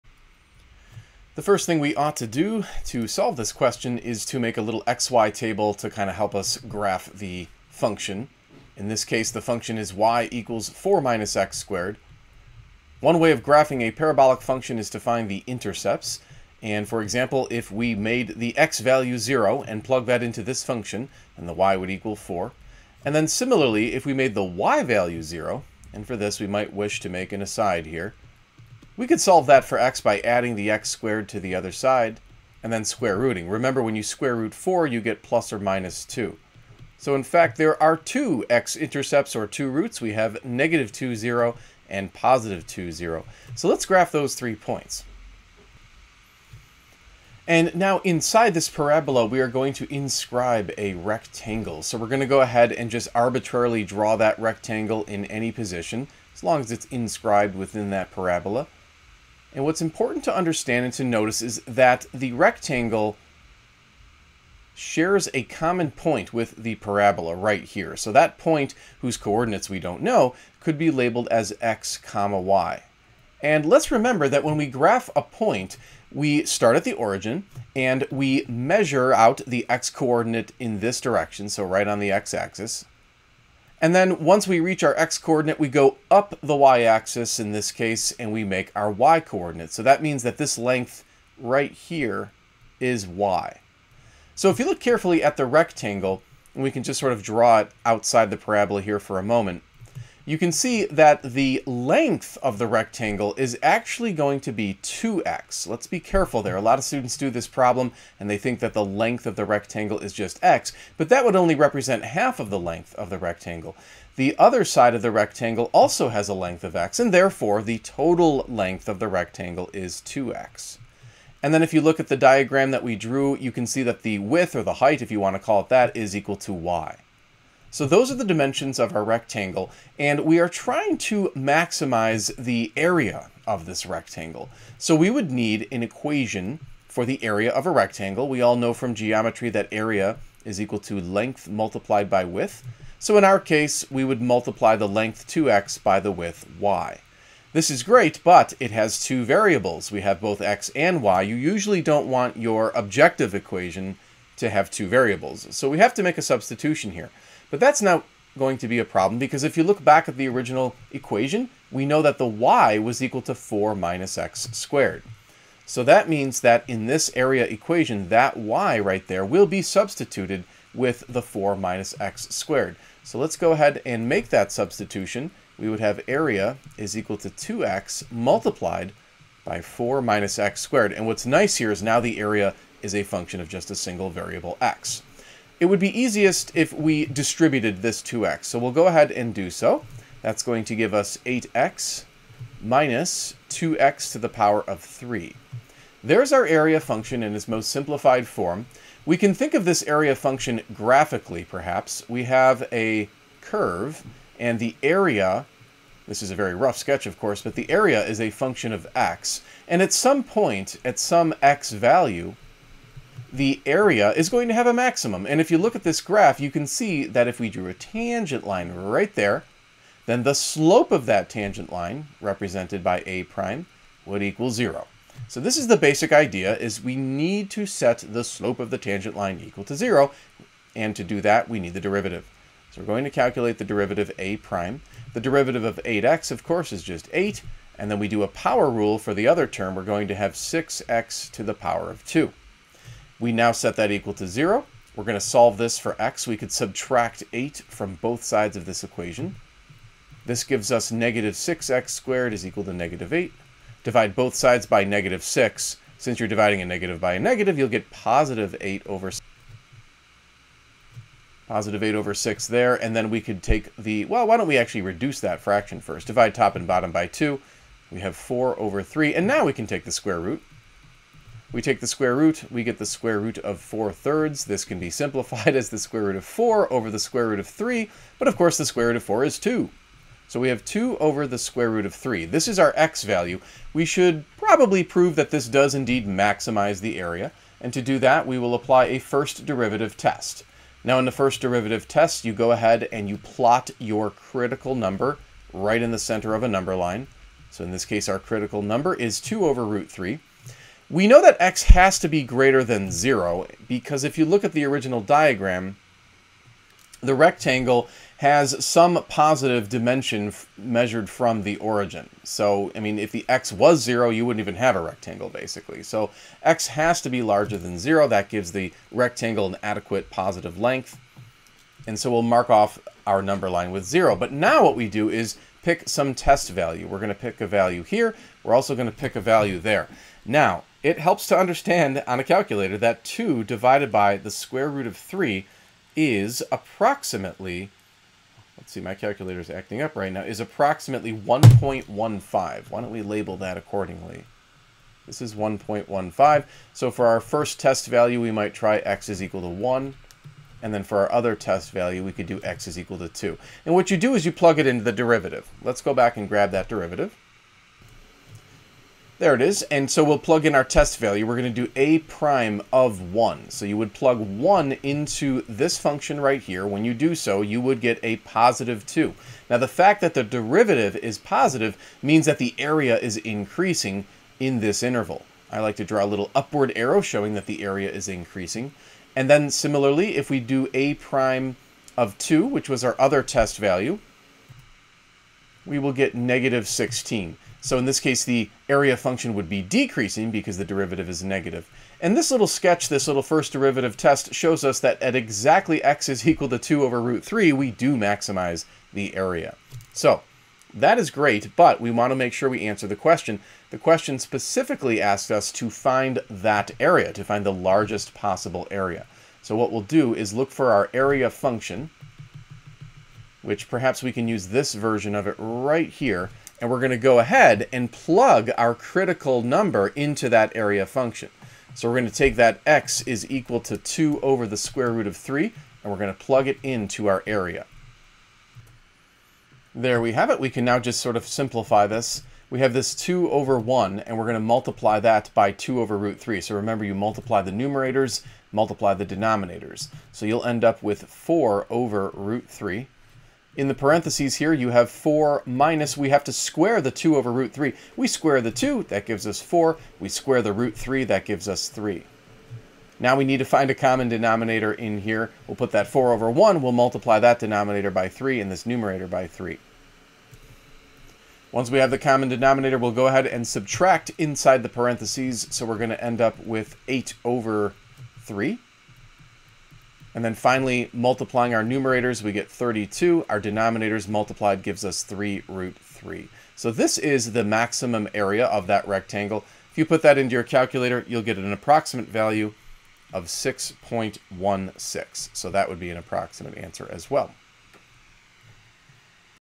A rectangle has its base on the x axis
A rectangle has its base on the x-axis and its upper two vertices on the parabola y = 4 - x^2. What is the largest possible area of the rectangle?